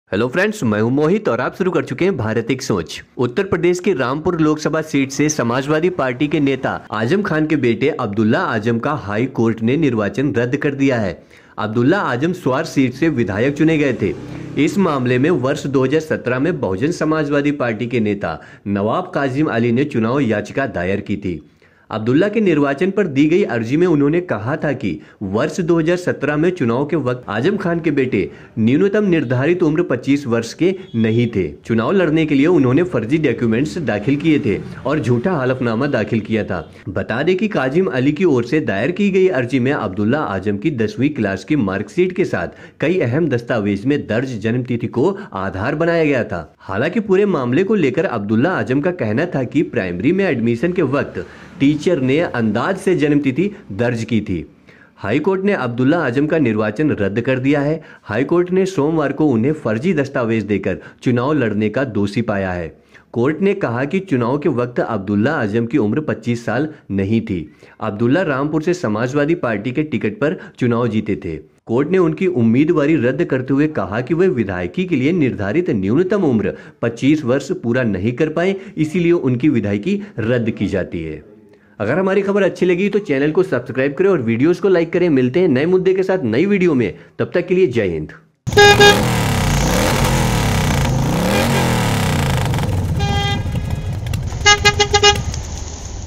Azam के बेटे की विधायकी रद्द
The High Court has canceled the election of Abdullah Azam, son of Samajwadi Party leader Azam Khan, from Rampur Lok Sabha seat in Uttar Pradesh.
Azam Khan
Azam Khan के बेटे Abdullah Azam Khan

क्लिक करिए और हमारे साथ जुड़े रहिए